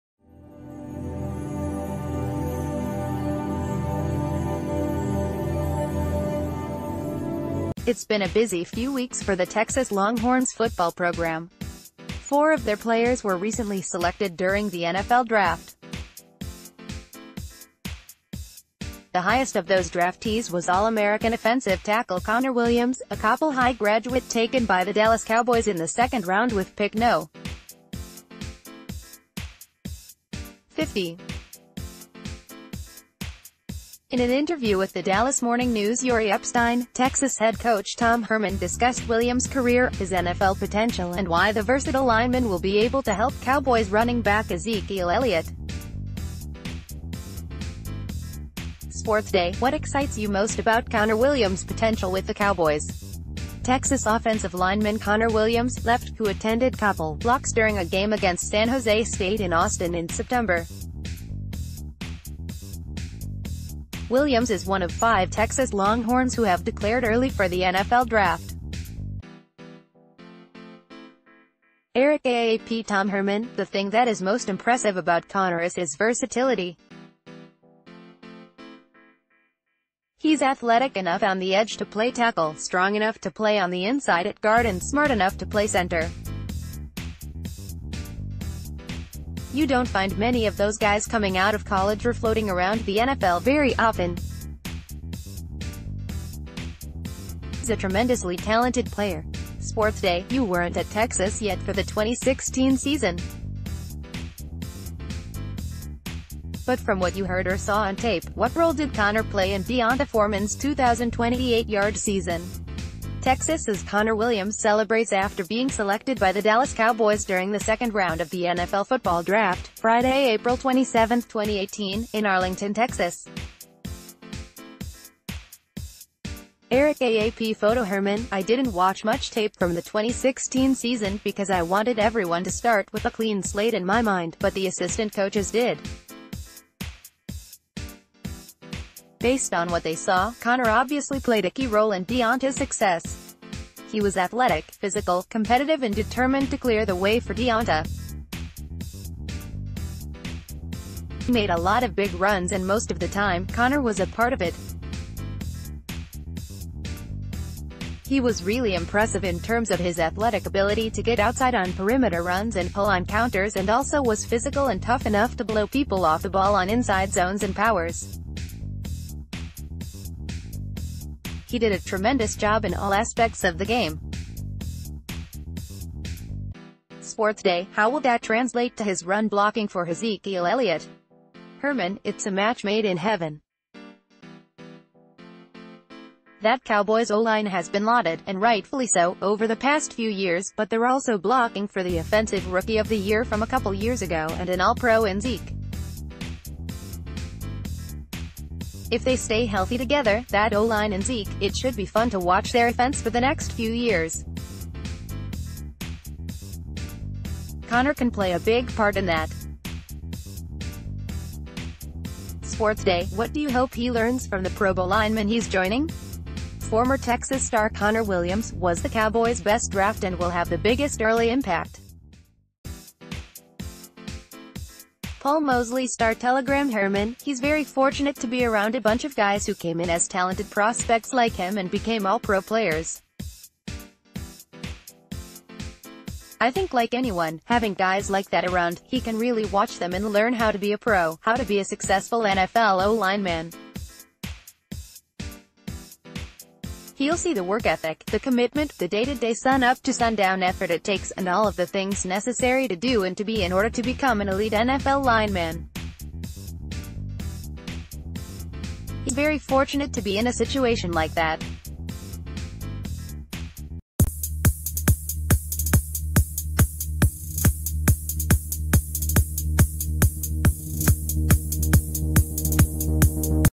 Breaking News | Tom Herman talks Ezekiel Elliot, Connor Williams and Cowboys
It's been a busy few weeks for the Texas Longhorns football program. Four of their players were recently selected during the NFL Draft. The highest of those draftees was All-American offensive tackle Connor Williams, a Coppell High graduate taken by the Dallas Cowboys in the second round with pick No. 50. In an interview with the Dallas Morning News' Jori Epstein, Texas head coach Tom Herman discussed Williams' career, his NFL potential and why the versatile lineman will be able to help Cowboys ...